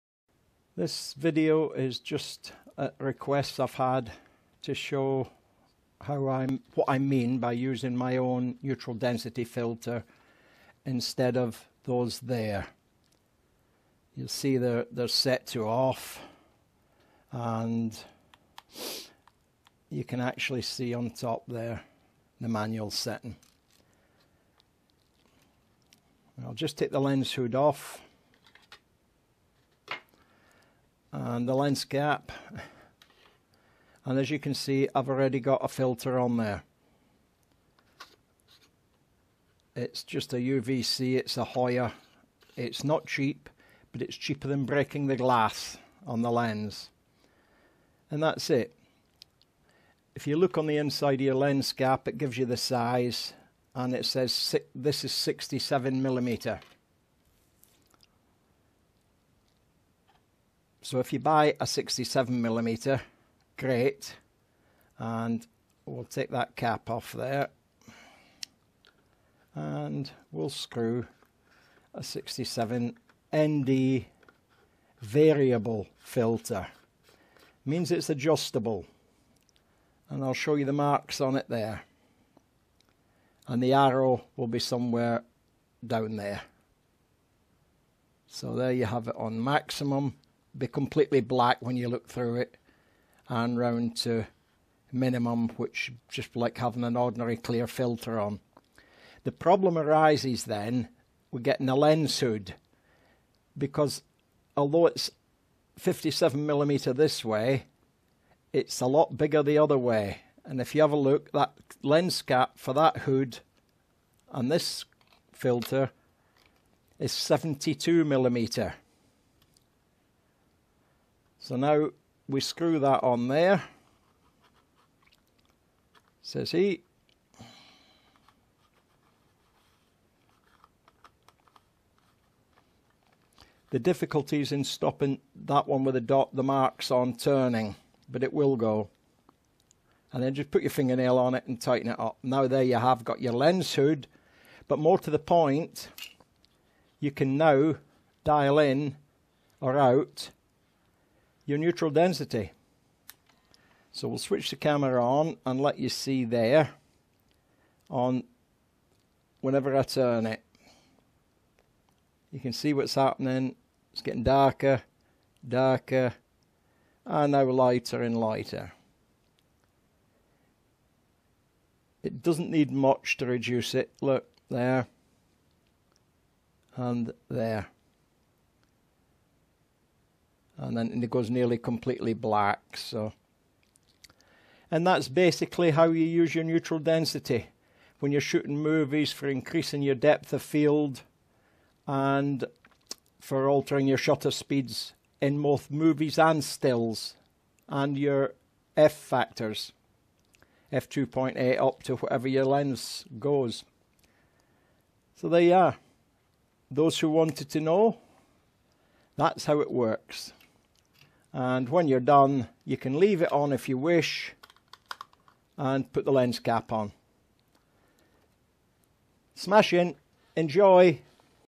FITTING A VARIABLE ND FILTER, HOOD AND CAP TO THE FZ2000.
I work in manual,  and prefer to shoot with my own amount of control on the ND filters. This way I can.
This way gives me infinite options, and total control over what my camera delivers for me.